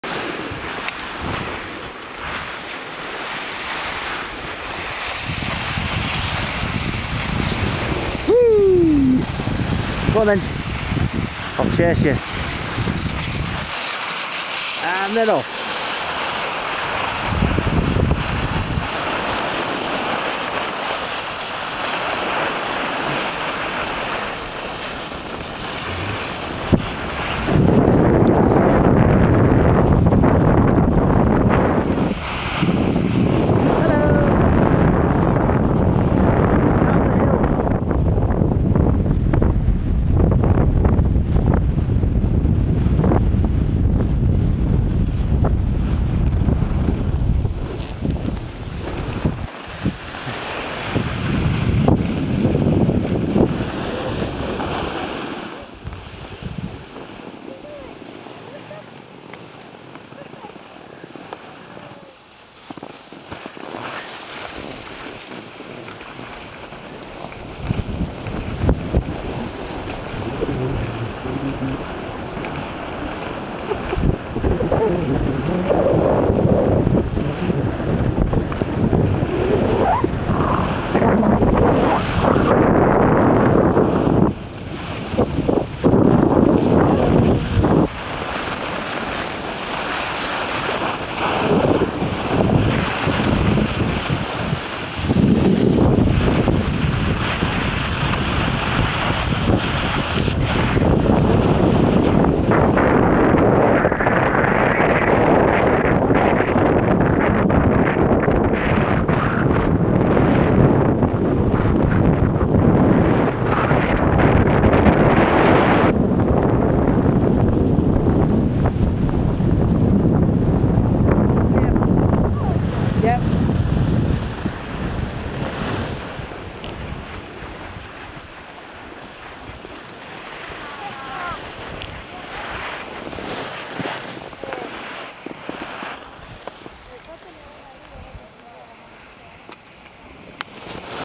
Little Fellas Montgenevre 2012
Kids getting a taste for speed.